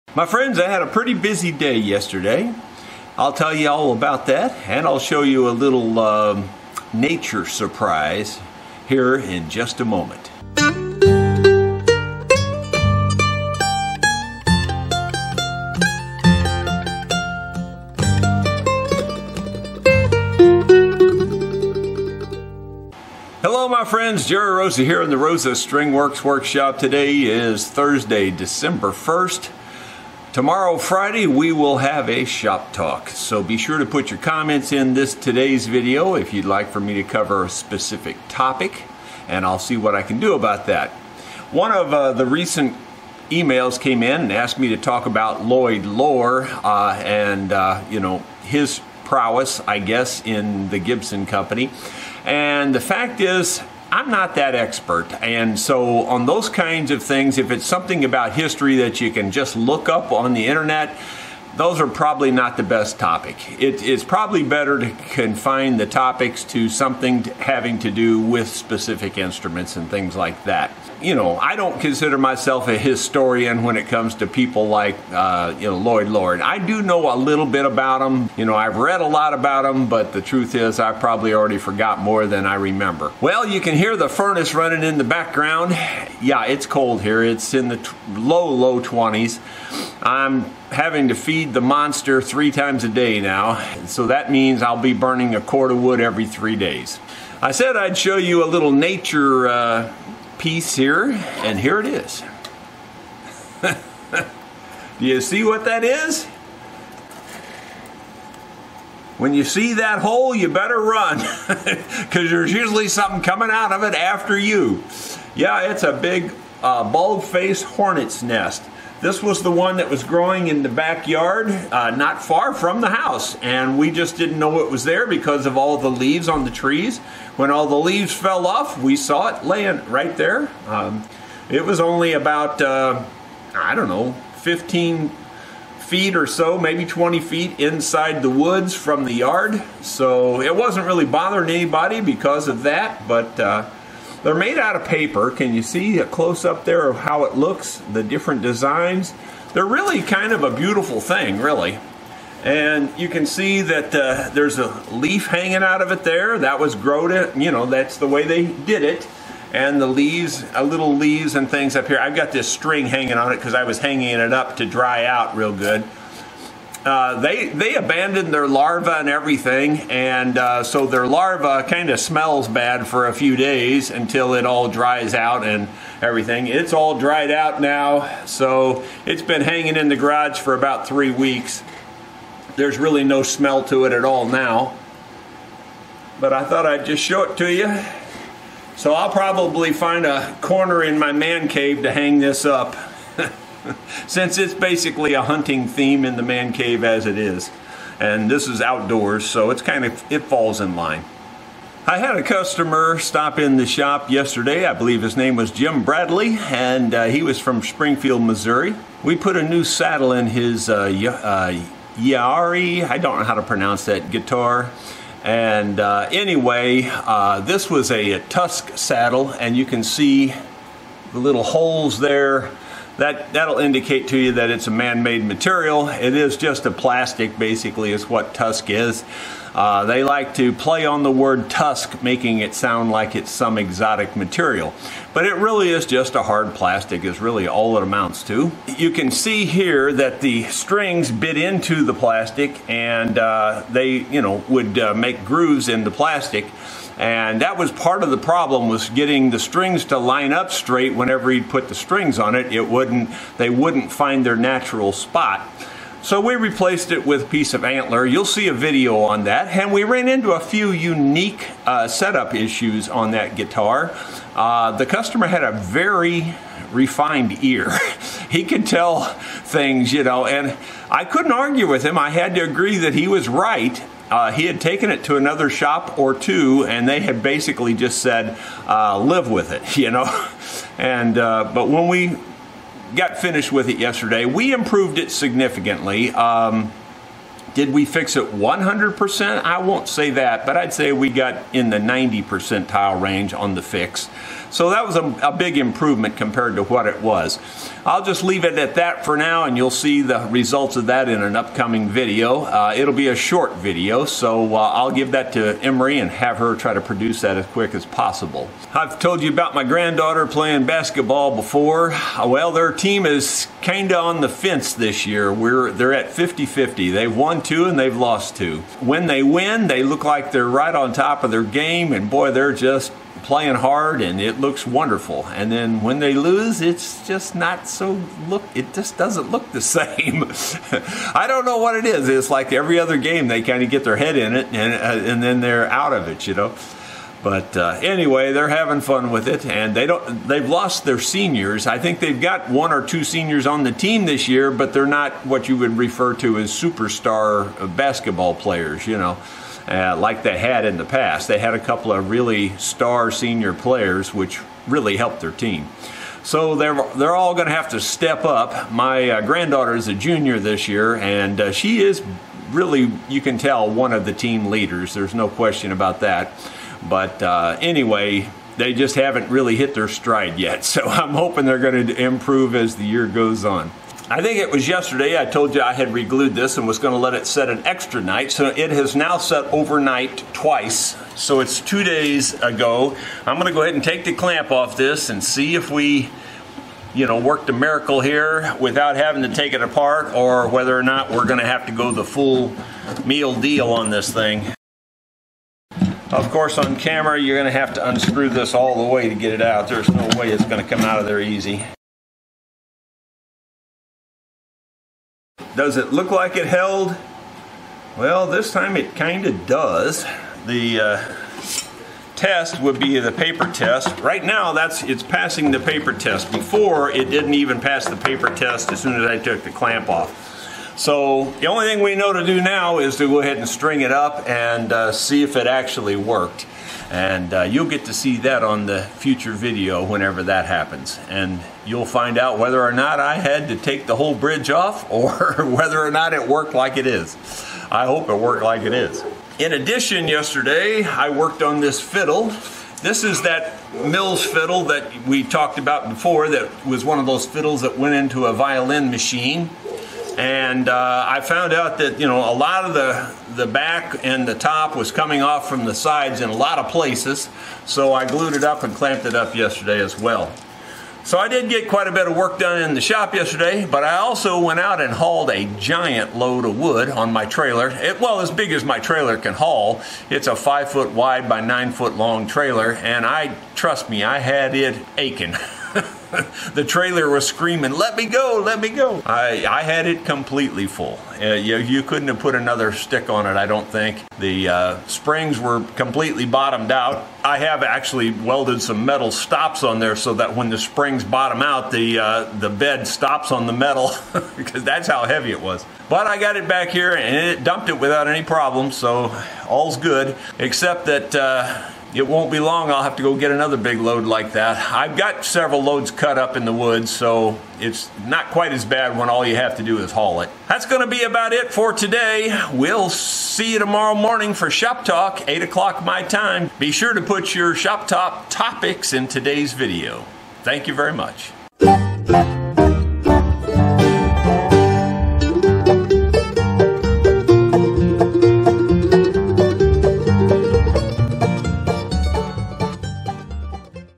Vlog #150 Gibson J45 and Mills Fiddle Status and More
Had a busy day working on the J45 and the Mills violin.  There is more to show as well.  Hope you enjoy!